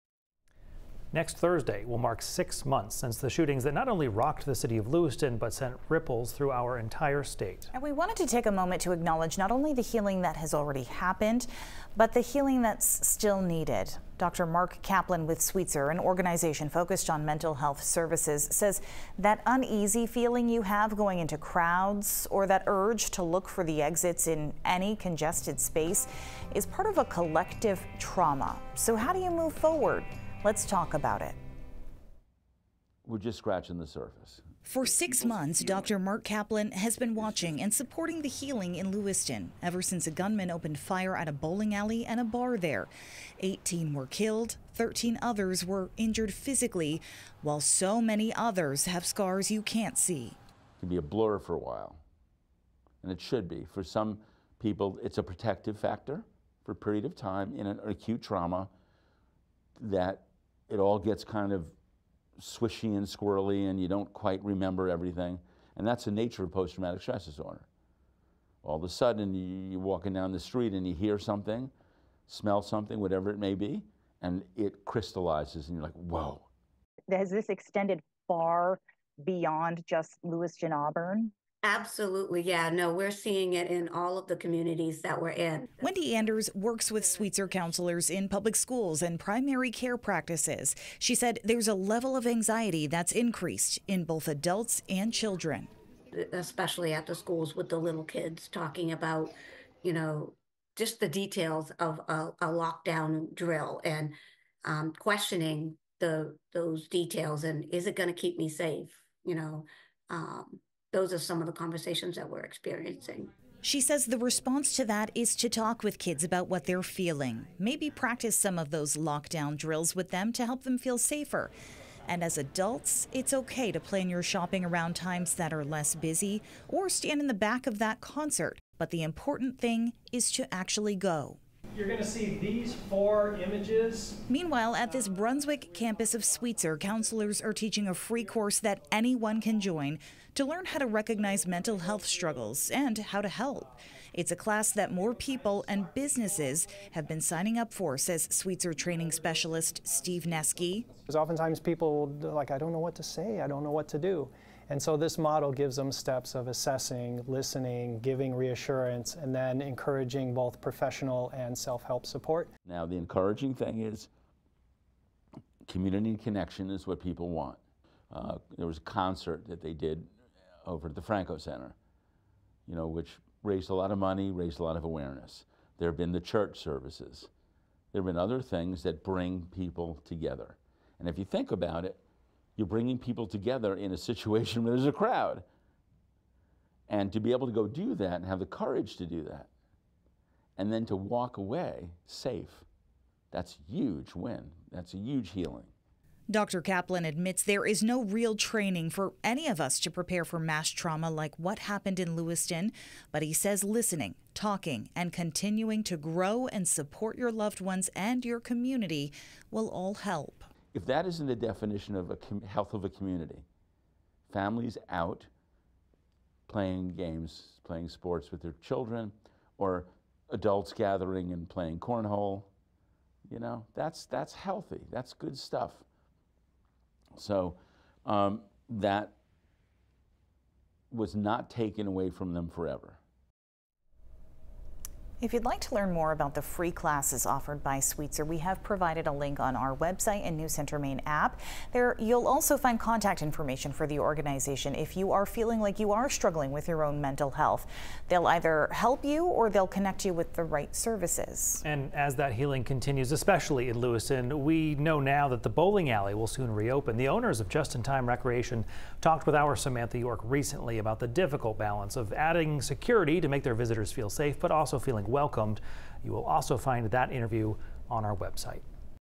Let's Talk About It: 'Collective Trauma' after Lewiston shootings and how to move forward
Dr. Marc Kaplan with Sweetser says donating, supporting, and being a part of the community is a healthy way of healing after trauma.